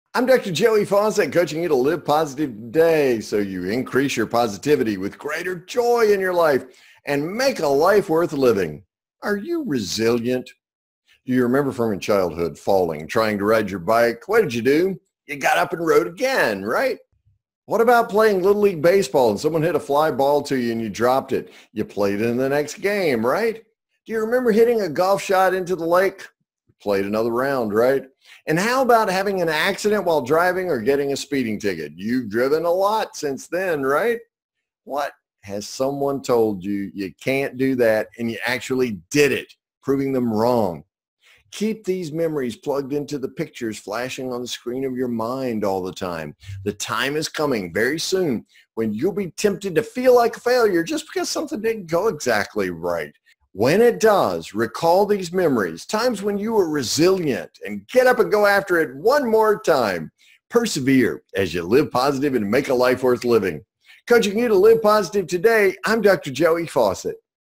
Are you resilient?
When you feel like a failure, recall times when you were resilient.  Then get up and go after it one more time.